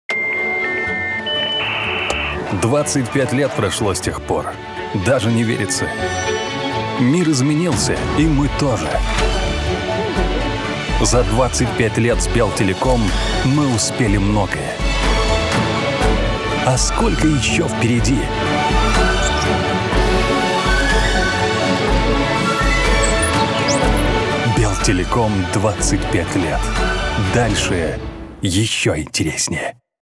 Рекламный ролик / Белтелеком | 25 лет
Весна 2020 года запомнится всем белорусам не только коронавирусом, но и ещё одним значимым событием.
Уже 25 лет компания «Белтелеком» работает и развивается для нашего комфортного общения.
ACTION STUDIO в дуэте с агенством ADDICTED  представляет рекламный ролик, приуроченный к юбилею компании.
Несмотря на сложную обстановку с пандемией, мы выполнили этот проект на высоком уровне.